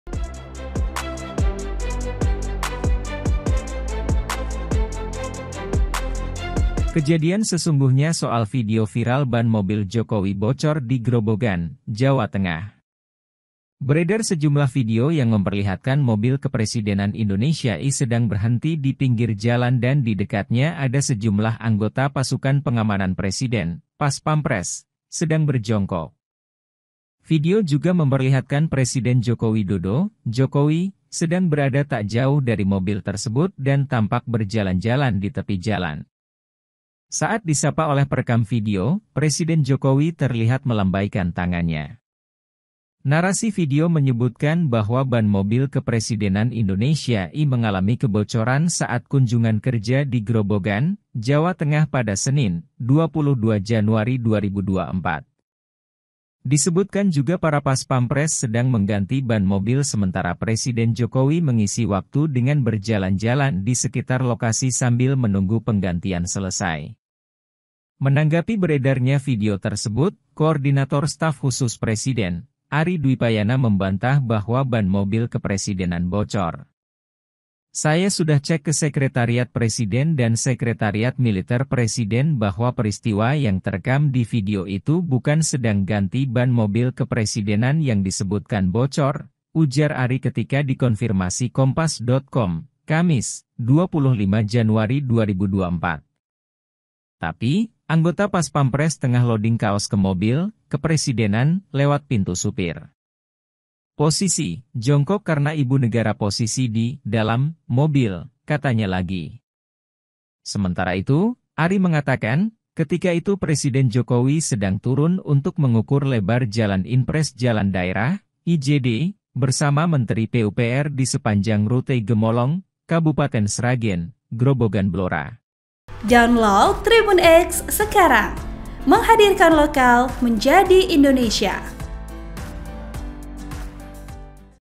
Istana Buka Suara Viral Ban Mobil Jokowi Diduga Bocor Kala Kunjungan ke Grobogan Jawa Tengah
BANGKAPOS.COM -

VP:Agus s